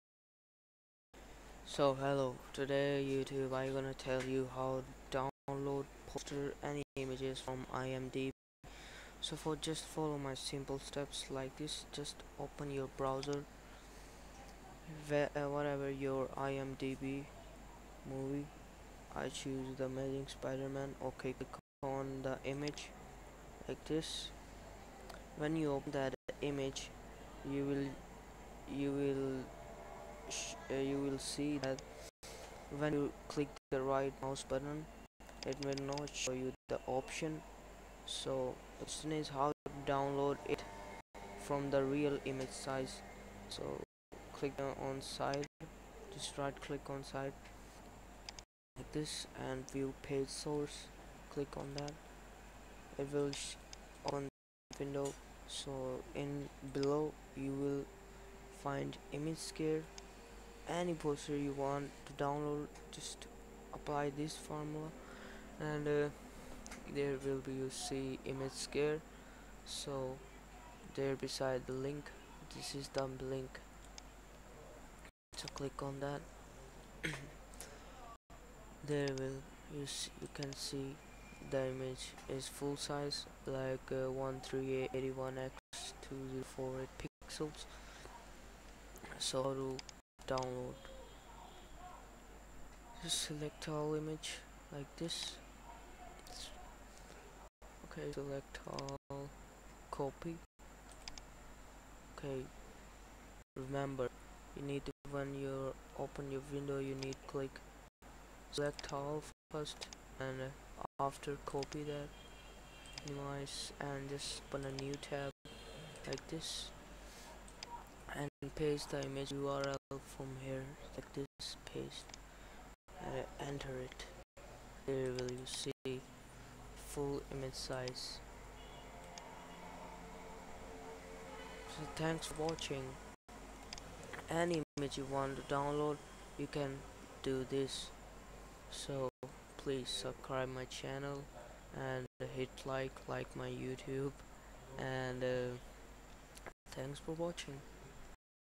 How To Download Images And Poster From IMDB Website Full Short Tutorial - HD+1080p
Title : How To Download Images And Poster From IMDB (Internet Movie Data Base) Website Full Short Tutorial - HD+1080p
Source From : IMDB ( Intenet Movie Data Base )
Steps :
1 - View Page Source
2 - When Window Open Select All Page
3 - After Copy And Paste into new tab
By : 9Lions Productions
2012 copyright Tut